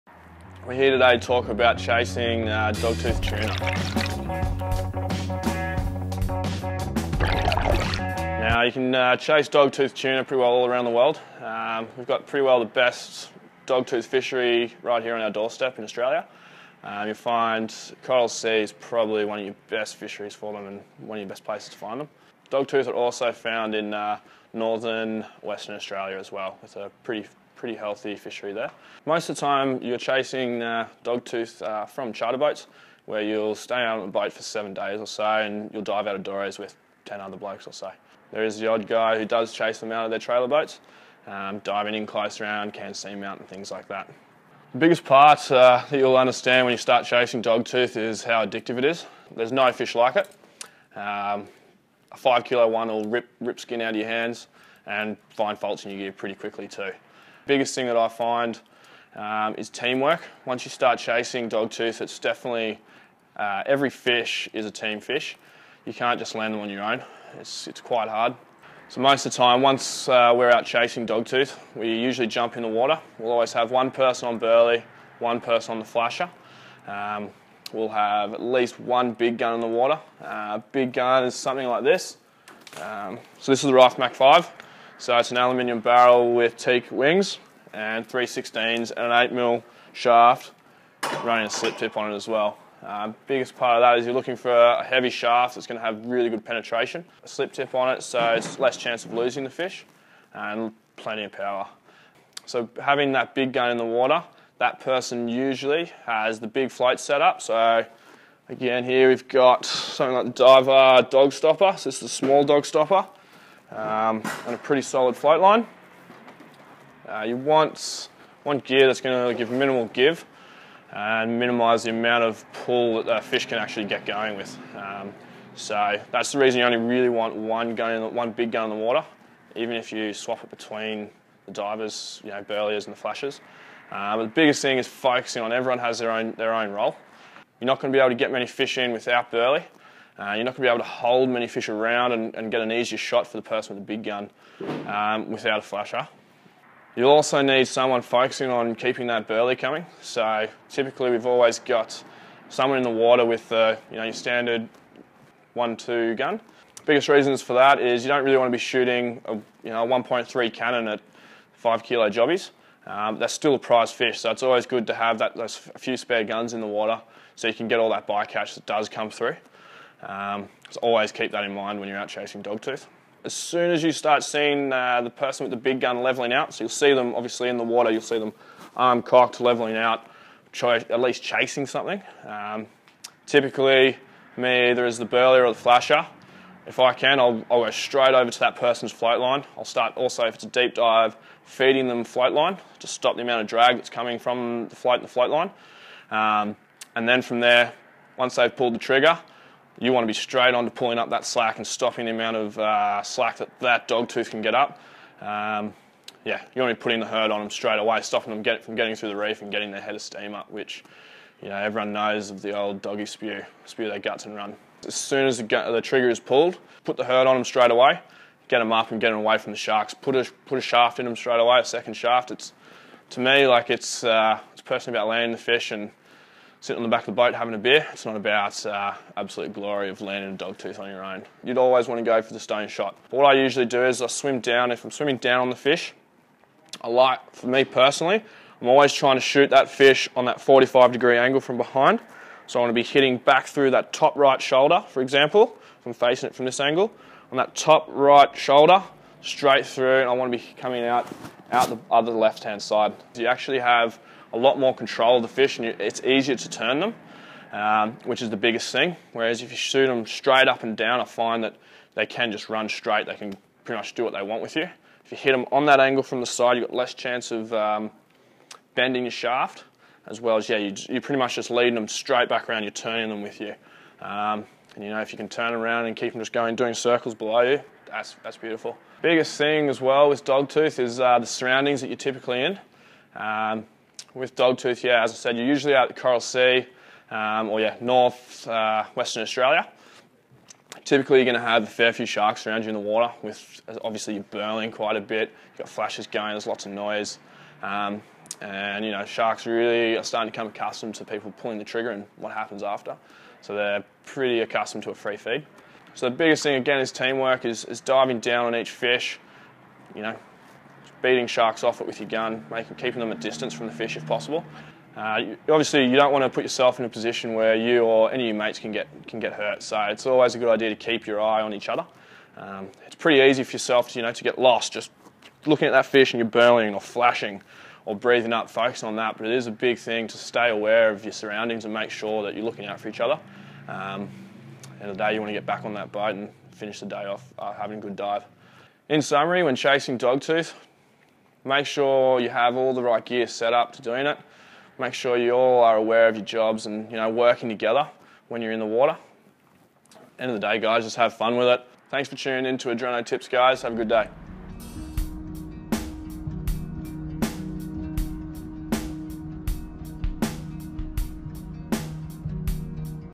Hunting Dogtooth Tuna | ADRENO
Spearfishing Dogtooth Tuna and everything you need to know. If you're looking for a challenge, then look no further than the Dogtooth Tuna. Dogtooth Tuna are renowned gear and skill testers. Make sure your spearfishing gear is in good nick. In this tips video, Taylor will tell you the ins and outs of landing a Dogtooth with your mates.

Dogtooth tuna are renowned for being one of the most addictive fish to hunt, where spearfishermen must compete with the speed and strength of the tuna. Any lacking experience, skill or quality equipment is going to show when you come up against one of these fun yet formidable catches. It’s important to have sufficient quality spearfishing gear all in good nick to land a dogtooth tuna. A reliable float and float line are imperative when hunting such a powerful fish.

Spearing doggies requires you to have a team to work with. There's nothing easy about spearing dogtooth tuna. There needs to be one person controlling burley distribution, one controlling the flasher and one person with a large powerful gun that can guarantee the success of the hunt. Without the constant attraction provided by both the burley and the flasher, fish aren't going to stay around very long and your chances of getting a decent shot at the fish are slim.

Taylor recommends that the spearer aims for the shoulder at an angle to impede the ability of the fish to run as much as possible and give yourself maximum control of the fish from the get-go and the best chance of landing the tuna. As the burley or flasher controller, your immediate reaction to the spearer below levelling out as they chase or shoot their target should be to go straight over to the float line and feed float line down in deep water to reduce drag. Once the shot is made, provide tension to the float line pulling the fish to the surface to prevent the dogtooth tuna gaining any momentum, reaching the reef or detaching from the spear. Once closer to the surface a second shaft should be placed into the fish to ensure the catch.

Always be aware of your environment. Be aware of the physical features and of any sharks looking for a free feed.

We hope you enjoy these dogtooth tuna hunting tips, provided to you by Taylor, one of our experienced members here at Adreno.